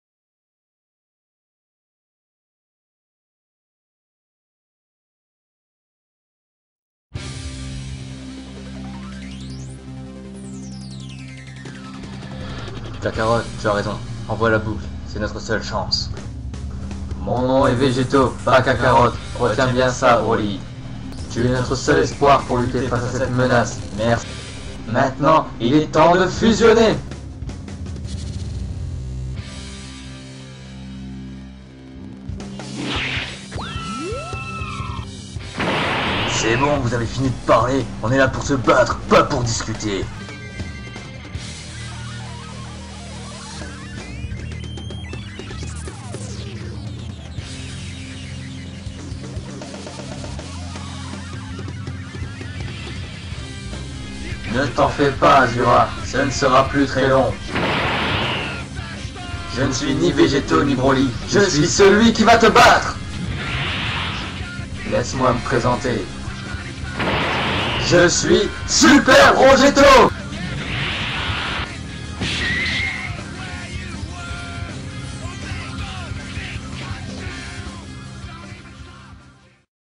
DBZ Fan Animation - Dragon Ball Wrath EP 1
Dragon ball Super fan animation - Dragon Ball Wrath EP 1  Dragon Ball Z | Asura's Wrath

Fan animation française sur l'univers de dragon ball et d'asura's wrath!